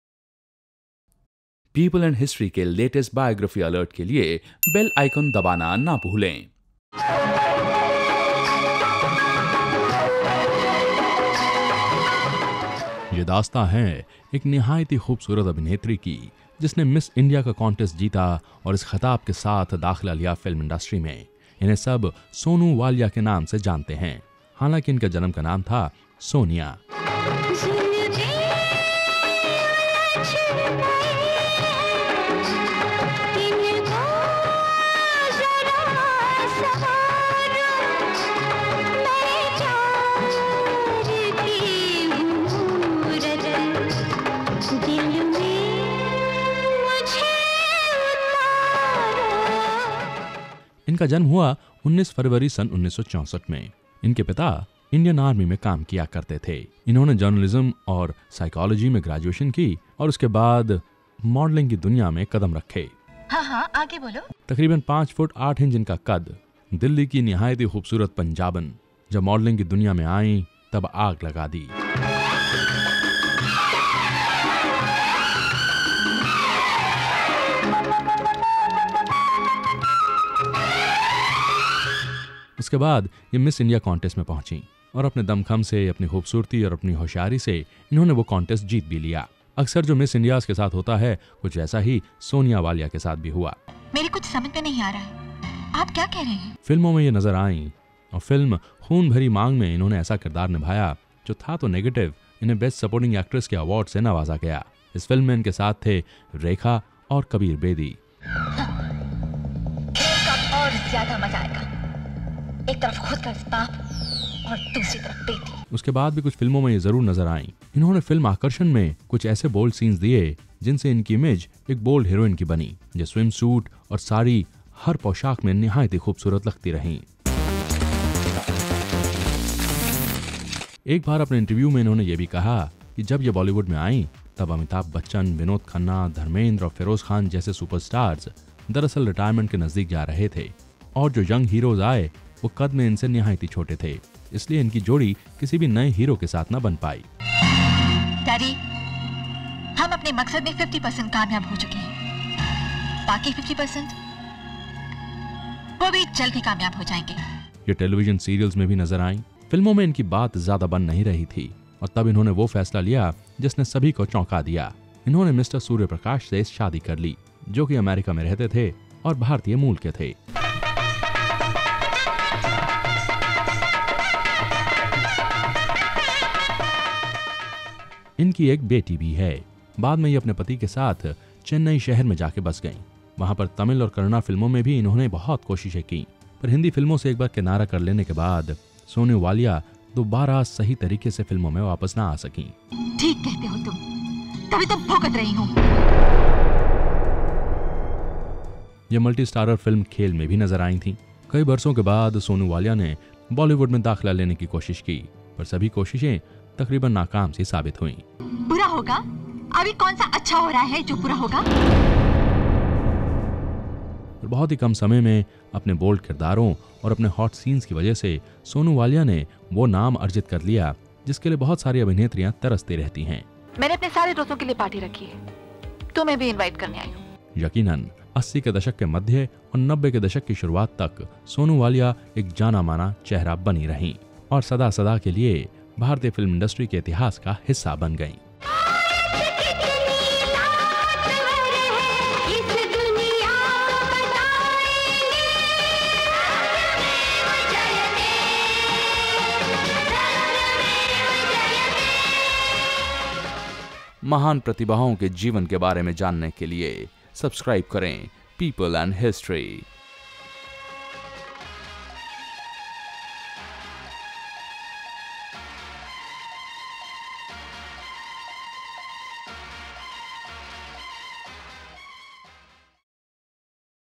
Sonu Walia - Biography in Hindi | Bollywood Actress | सोनू वालिया की जीवनी | बॉलीवुड अभिनेत्री
Watch Sonu Walia Biography in Hindi and Know about Her Life Story and Unknown Facts.